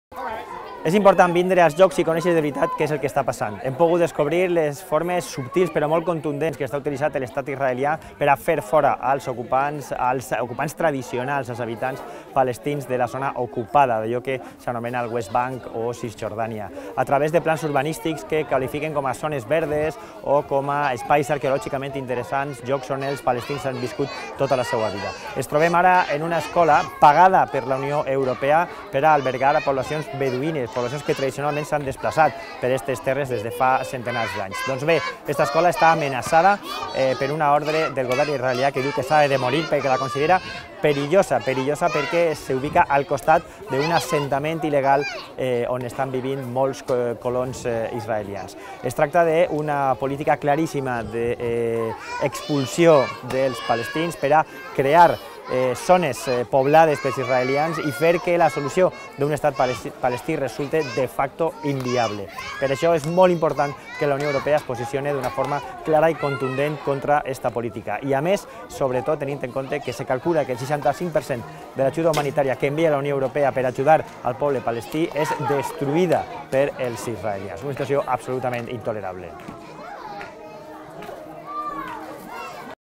Sebastià: "Europa ha de prendre partit per la pau a Palestina"
L'eurodiputat de Compromís-Primavera Europea ens envia una crònica des dels territoris ocupats de Palestina.